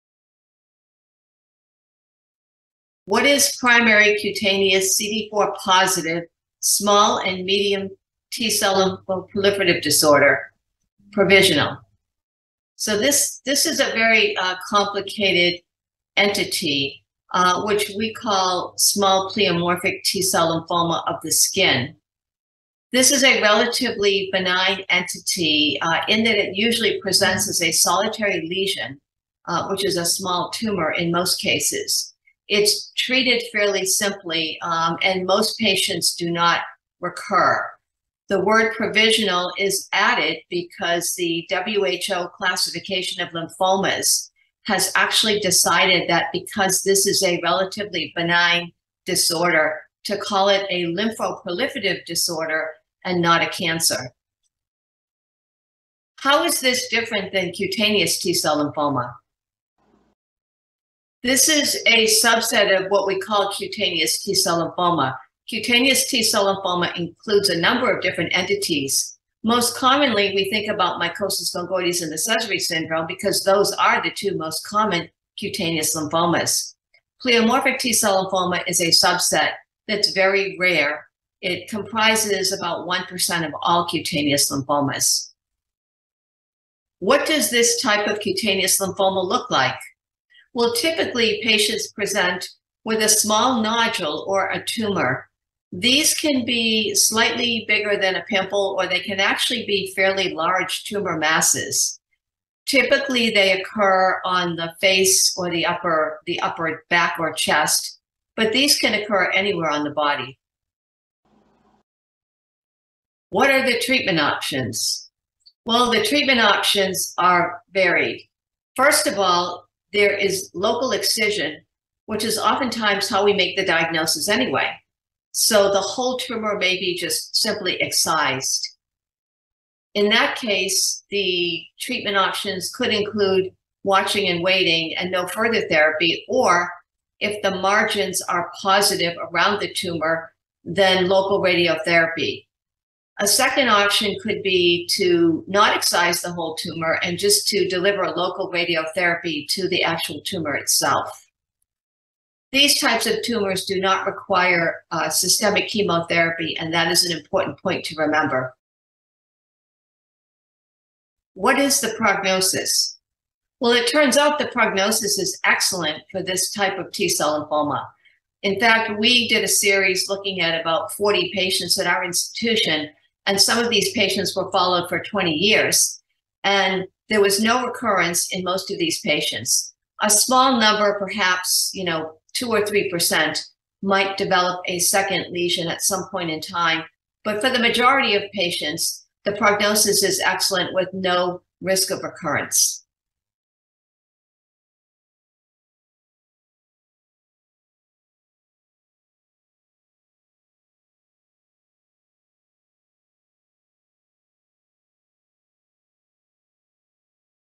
Rare Variant: Primary cutaneous CD4+ small/medium T-cell lymphoproliferative disorder (provisional)
We've asked cutaneous lymphoma expert Dr. Francine Foss to respond to these questions regarding this rare variant of cutaneous T-cell lymphoma:
How is this different from other types of cutaneous T-cell lymphoma?
What does this type of cutaneous lymphoma look like?
What are the treatment options?
What is the prognosis?